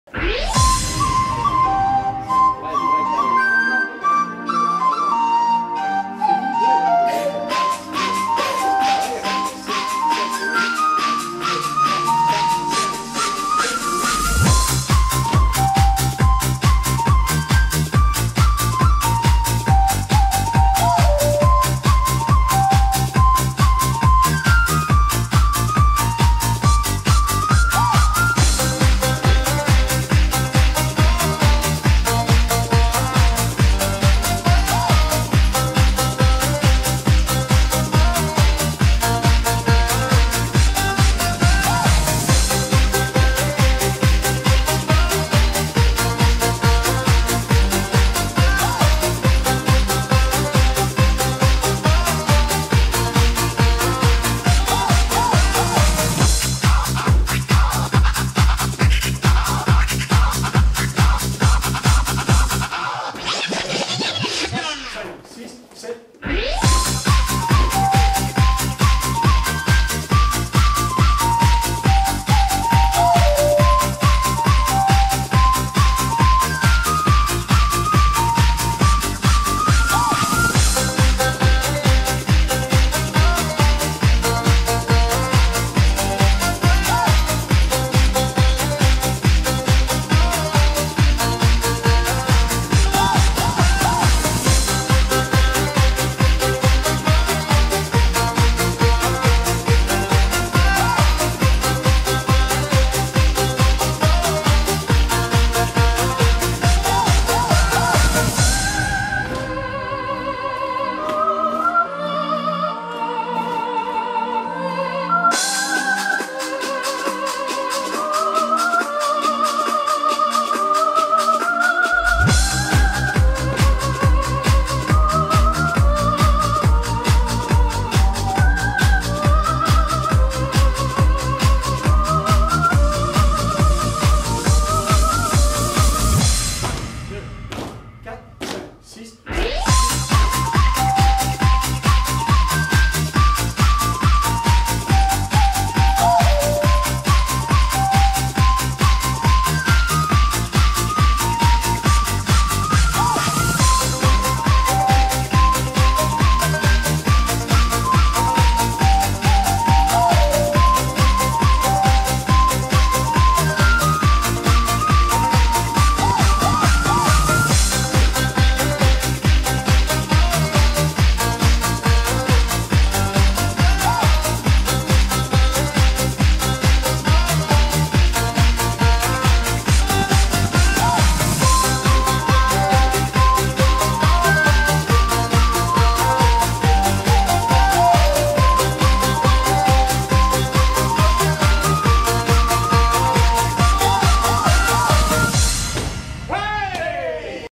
Mini Flute Démonstration
Danse Celtique Electro en Ligne de 32 temps sur 4 murs de niveau Débutant avec 1 Restart et 1 TAG
Chorégraphe : Marie Sorensen ( Danemark 09/2011 )